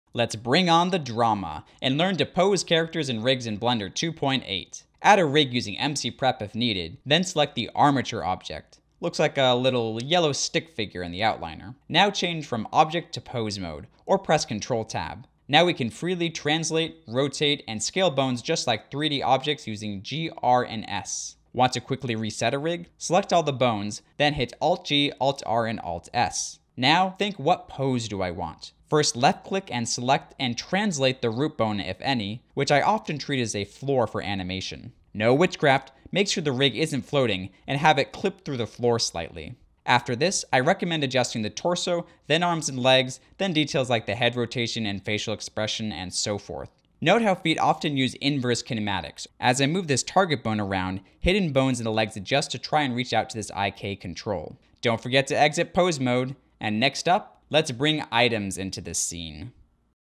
How to Pose Rigs in 1 minute | Blender 2.8 Minecraft Animation Tutorial #8
Learn how to adjust the bones of an armature to pose any character or mob rig, with some quick practical tips!

Moo-Ack!
~Patrick W. Crawford